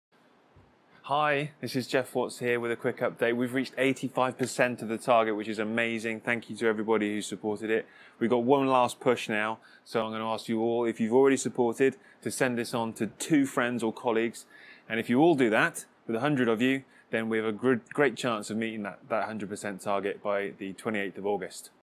85% and a Challenge
I have hit 85% of my Kickstarter target and I have a challenge for my backers.